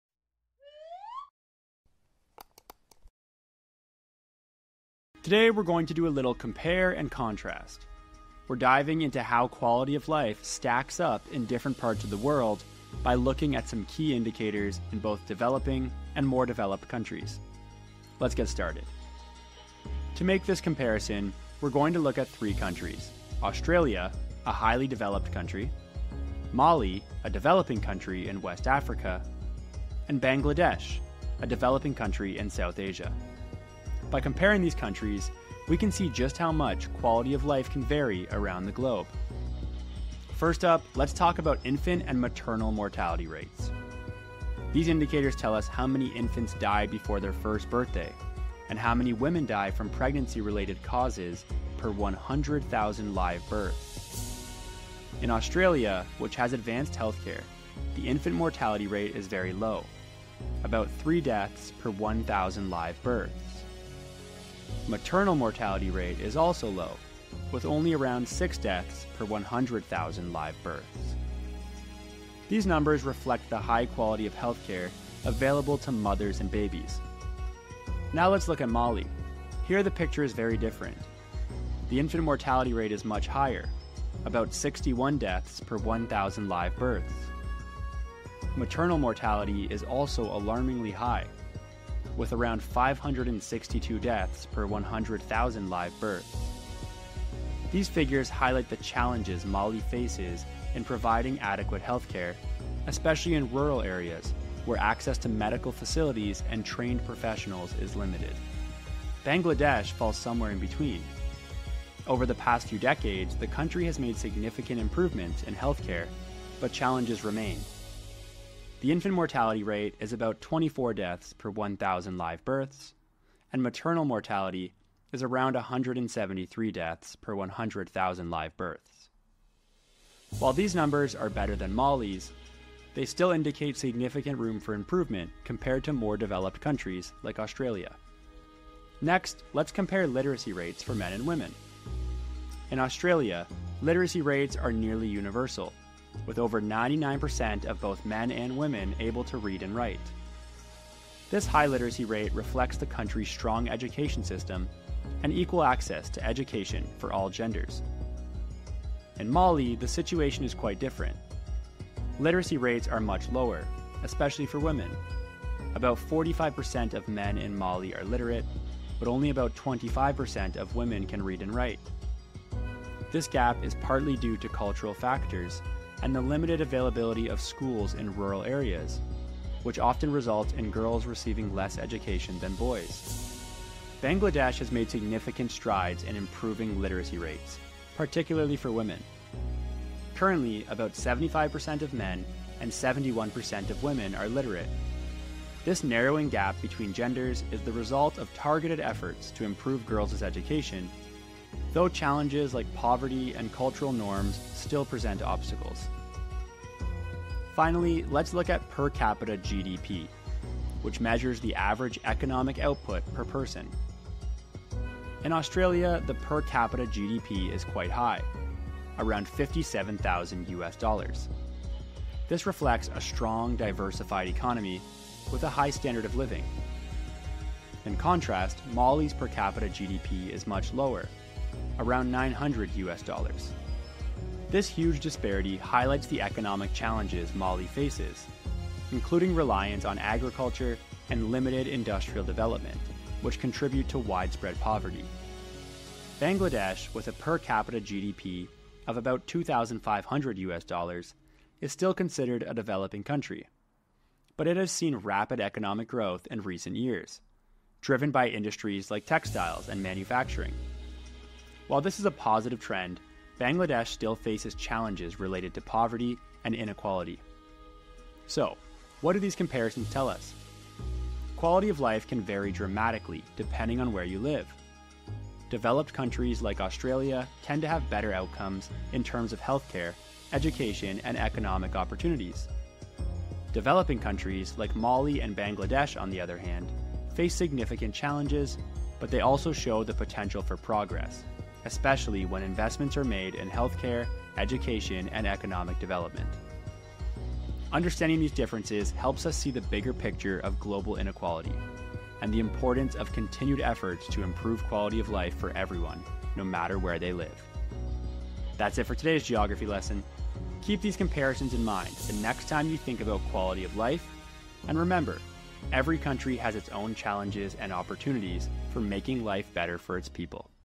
Comparing Quality of Life Indicators in Developing and Developed Countries
How does quality of life differ between developed and developing countries? In this lesson, we compare key indicators—like infant and maternal mortality rates, literacy rates, and per capita GDP—in Australia, Mali, and Bangladesh.

Learn how healthcare, education, and economic opportunities shape life in these countries and uncover the challenges and progress each one faces.

Dive in to understand the global disparities and the steps needed to improve quality of life for all!